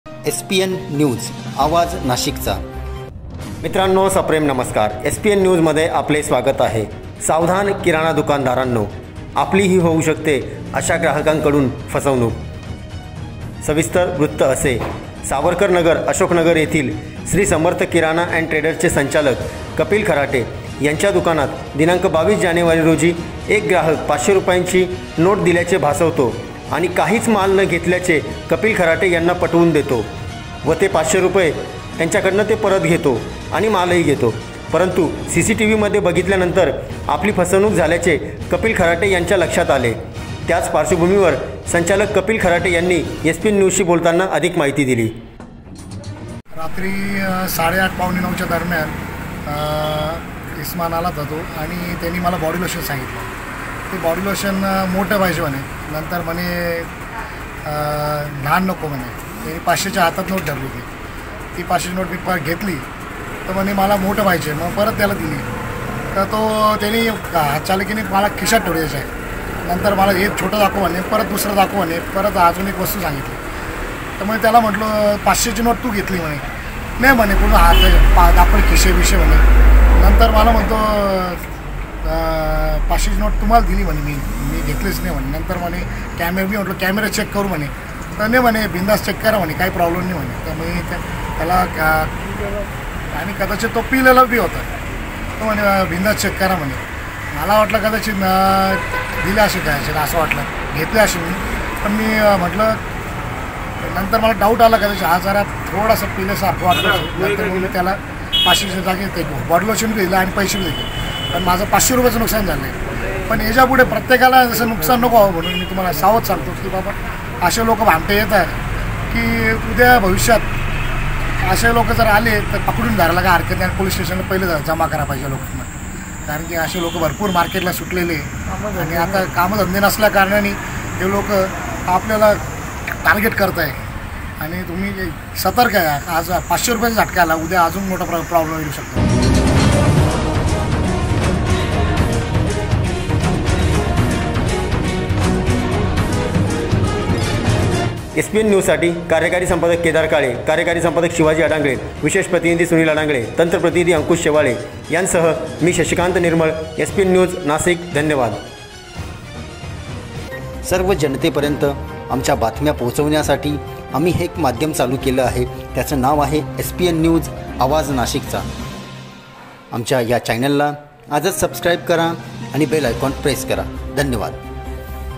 सावधान किराणा दुकानदारांनो आपली होऊ शकते अशा ग्राहकांकडून फसवणूक.
सावधान किराणा दुकानदारांनो आपली होऊ शकते अशा ग्राहकांकडून फसवणूक. सावरकर नगर,अशोकनगर येथील प्रकार.